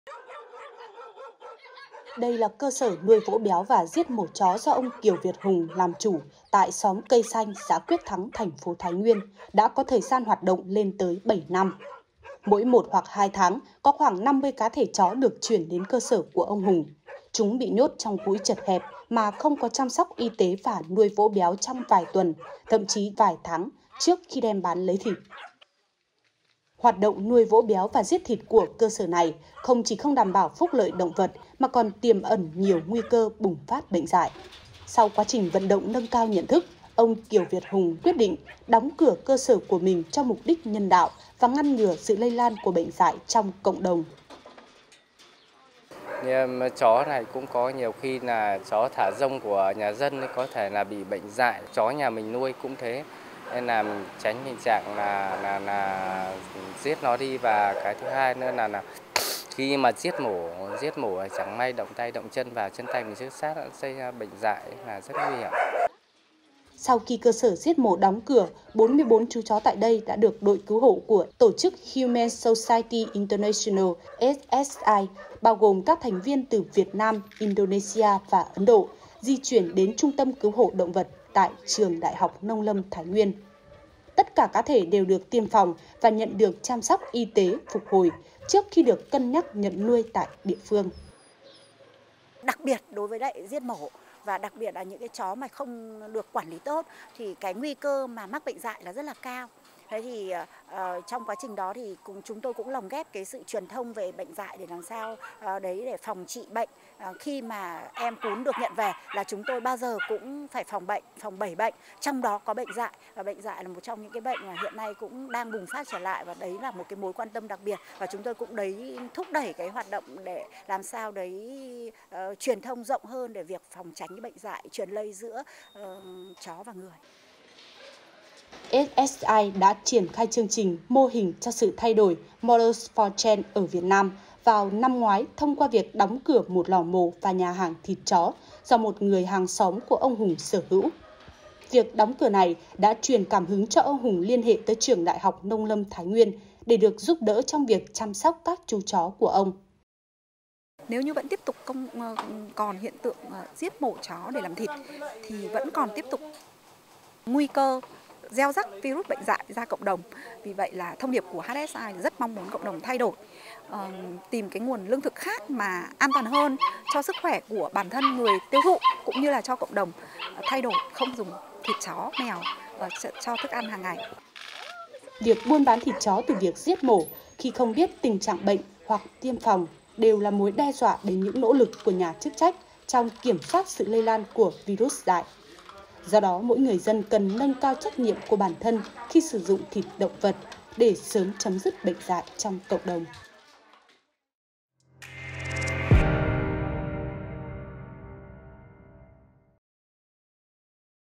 Ngừng giết mổ chó, phòng tránh bệnh dại lây truyền giữa chó và người
Tin tức mới nhất | Trước nguy cơ bùng phát bệnh dại, ông Kiều Việt Hùng quyết định đóng cửa cơ sở giết mổ chó sau 7 năm hoạt động ở xóm Cây Xanh, xã Quyết Thắng, thành phố Thái Nguyên.